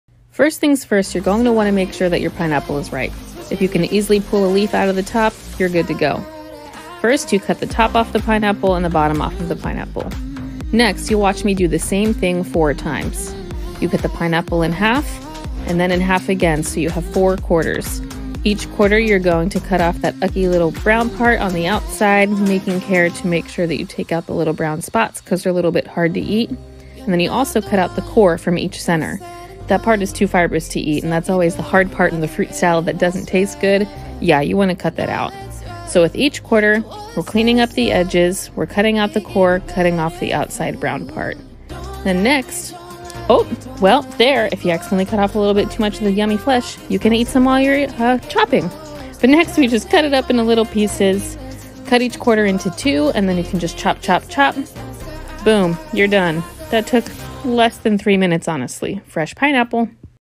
How to Cut Fresh Pineapple
Start your journey to a healthier lifestyle with FORM! Registered Dietitian Becca shows you how to cut a fresh pineapple. Making small changes, like incorporating fruits and vegetables into your diet, can make a big difference!